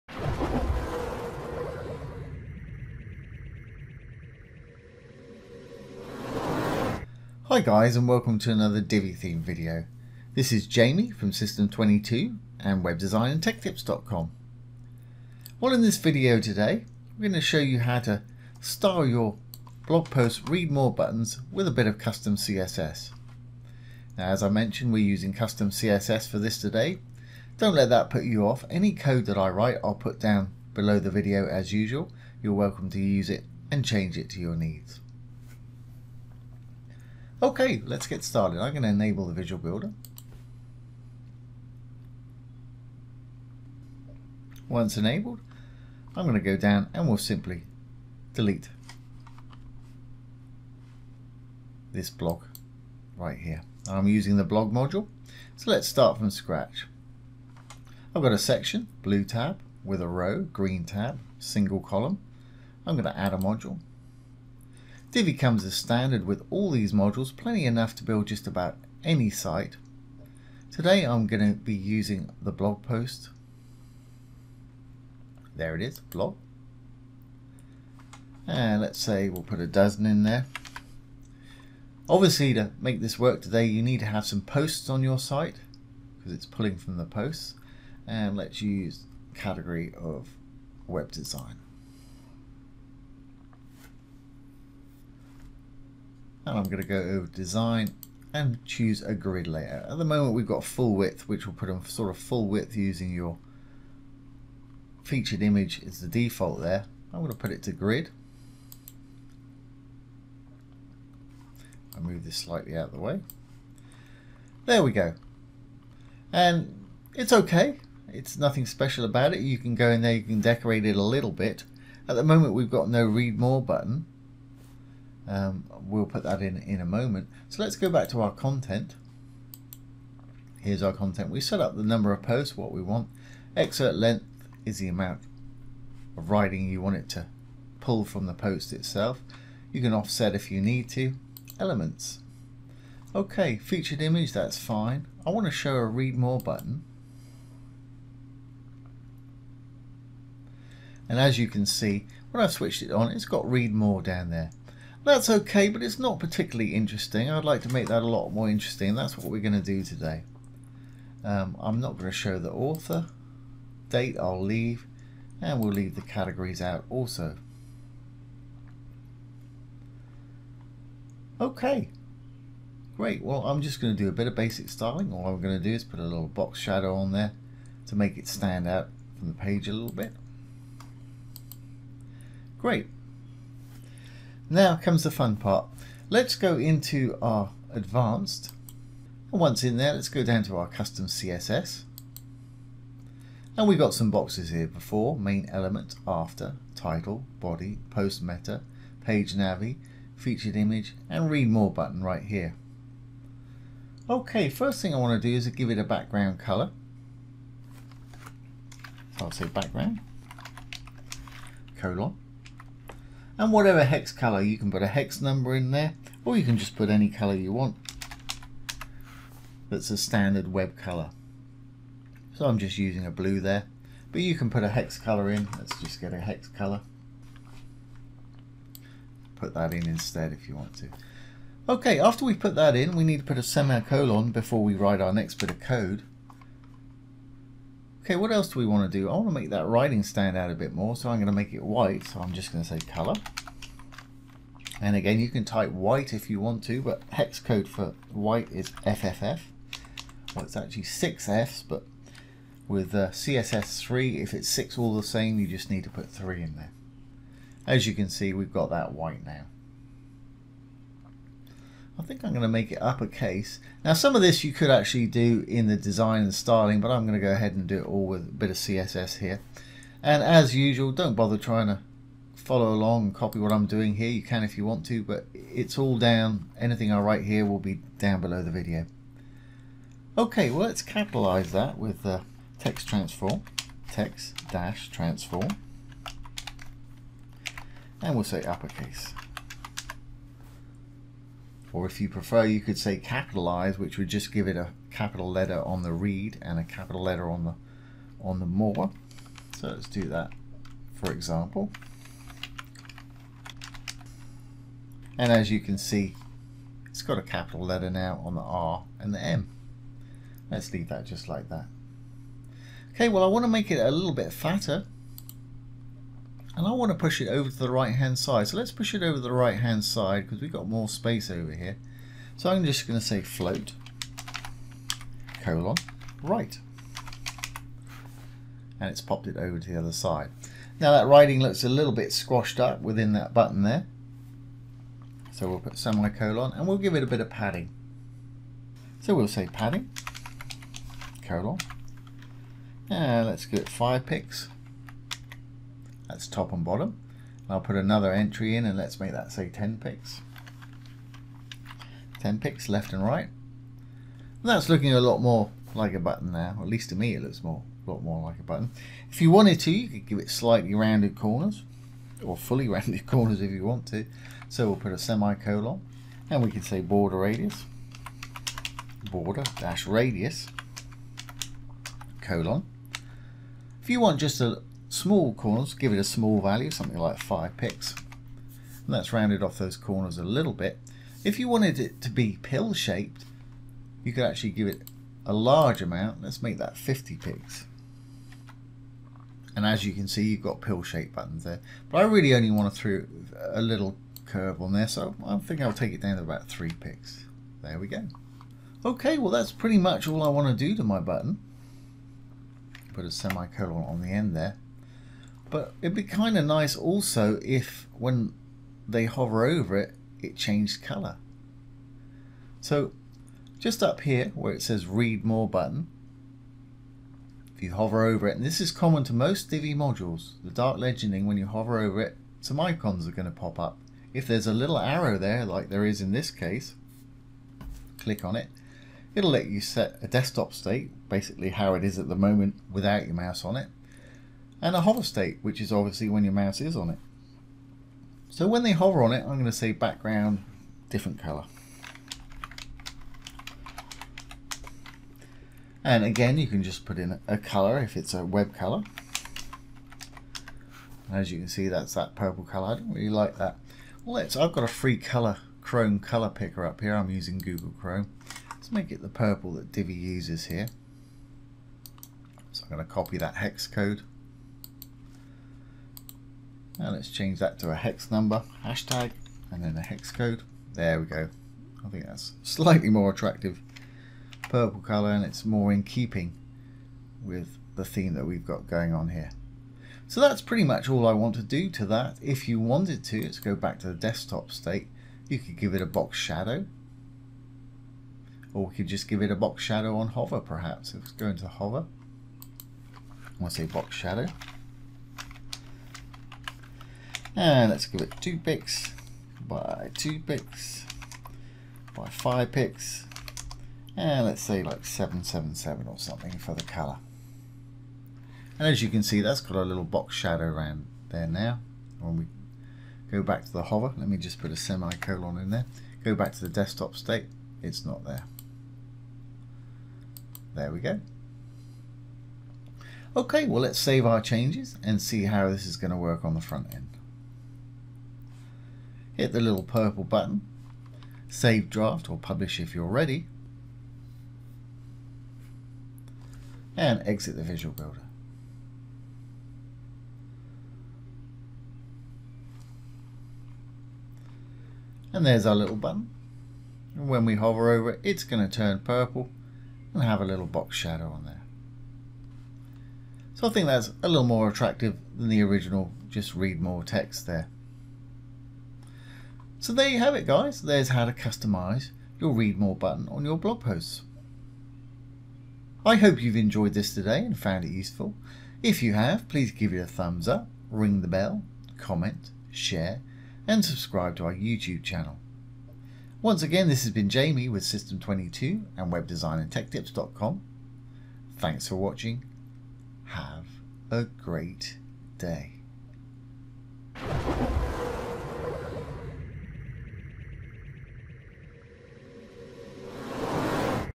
Divi Theme Style Your Blog Read More Button With CSS 👈👈👈👍
Divi Theme Style Your Blog Read More Button With CSS. We are going to be using the Divi theme to create some great effects in this series of videos. The Divi theme has some great modules and effects. With a little work you can achieve some great eye-catching effects to enhance the look and user experience of your website
Today we will be demonstrating how to style your Divi theme blog read more button using some custom CSS code. We are going to use the Divi blog module for this today. One the module is set up to show the desired posts, we will add the read more button. Using the Custom CSS box under the advanced tab, we will write some simple CSS code to change the button from lower case text, into a button that will change color on hover. This is very easy to do and will make the blog post grid look much more interesting.
In this video today, we are going to use a bit of CSS code to style the read more button the way that we want it, so it will better match the rest of the site. Don't let the CSS coding put you off, it is very easy, and any code I write today I will paste below the video for you to use as you wish. You are welcome to copy the code and paste it into your site, CSS is a great thing to learn, so I encourage you to try out some of your own ideas to get familiar with it. You can't break your site with CSS, if something doesn't work simply delete it to return back to how the site was previously.
So, follow along with the video and see how easy it is to style your Divi theme blog read more button using some custom CSS code, on a Divi website.

CSS CODE USED TODAY:

background:0c71c3;
color:fff;
text-transform:capitalize;
float:right;
padding:5px 10px;
border-radius:3px;

/* Hover State  */

background:6C2EB9;
box-shadow:2px 2px 5px #777;

CHAPTERS

 00:00    Intro
 01:06    Add Divi Blog Module
 01:29    Select Categories
 01:39    Select Blog Grid Layout
 02:22    Add read More Button
 03:10    Add Custom CSS Code
 06:58    Add Hover CSS Code
 09:00    Result

SOCIAL MEDIA
Follow what I'm doing on: